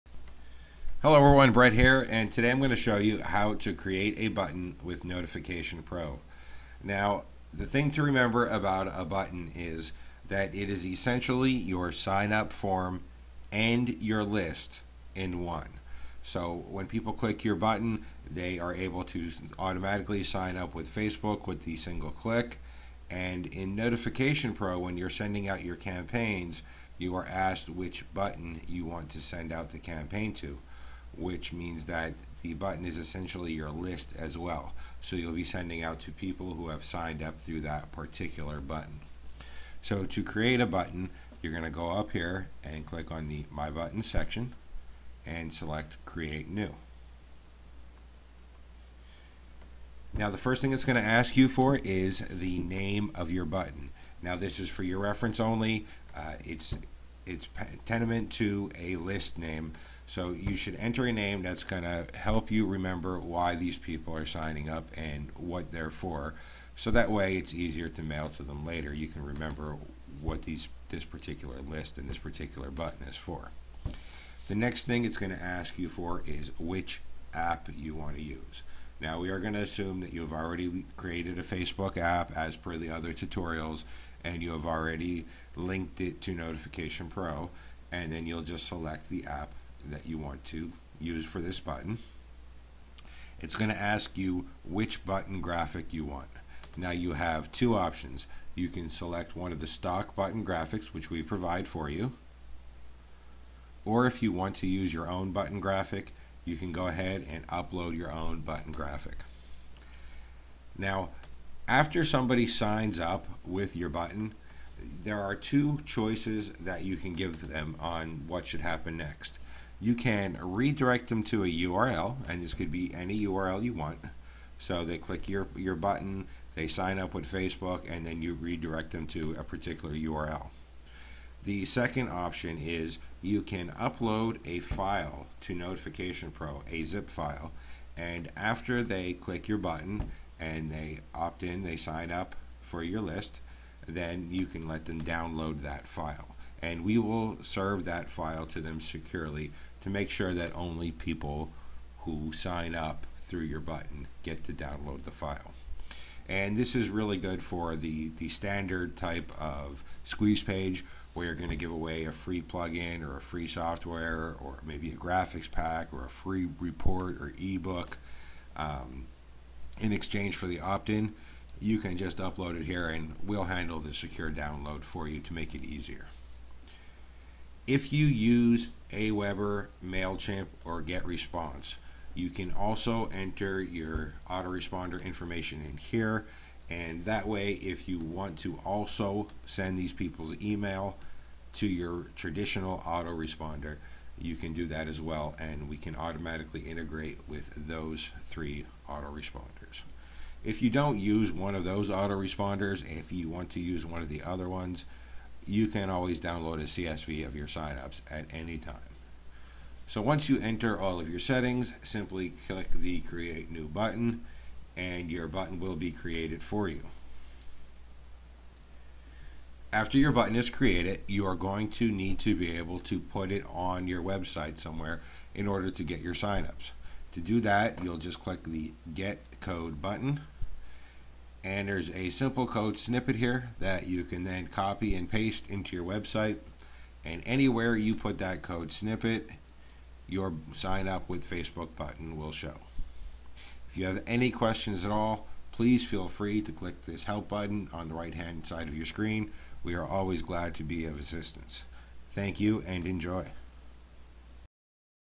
How to create a button with Notification Pro
Buttons are both your one click Signup with Facebook opt in form and also your list.